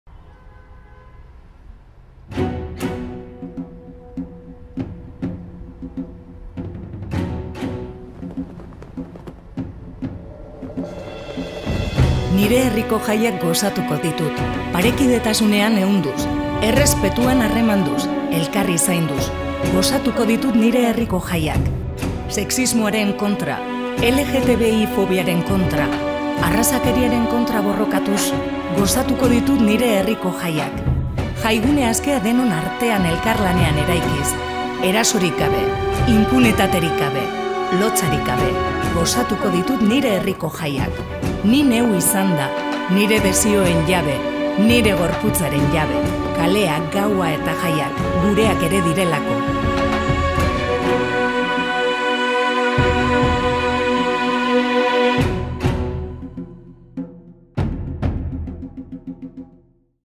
MARRAZTU DEZAGUN ERASORIK GABEKO JAIGUNE ASKEA
NIK GOZATU, ZUK ERRESPETATU, HURA ZAINDU

Azpitituluak euskaraz // Subtítulos Castellano // English subtitles

Erasorik gabeko Aste Nagusia nahi dugu, emakumeok aske gozatuko dugun Aste Nagusia nahi dugu.
Gure gorputzak eta gure erabakiak errespetatuko diren jaien parte izan nahi dugu.

Erasorik gabeko jaigune aske bat marraztuko dugu, lasai eta eroso sentituko garen jaigunea eraikiko dugu denon artean.
Emakumeoi ukatutako gozamena gure egingo dugu eta defendatuko dugu gure arteko sareak indartuz, gu geu izanda , lotsarik gabe, autodefentsa landuz eta errespetuzko jaigiroa behar den guztietan aldarrikatuz lortuko dugu.

Las fiestas están en la centralidad de la vida, porque conectan con el mundo de las ideas y de los valores, con la estructura social y con las relaciones entre las personas en general y entre las mujeres y los hombres en particular.

Es por ello imprescindible actuar frente a las actitudes y agresiones sexistas que coartan la autonomía de las mujeres en el disfrute pleno y legítimo de las fiestas, que también son nuestras.

AZPITITULUAK EUSKARAZ
SUBTÍTULOS EN CASTELLANO
ENGLISH SUBTITLES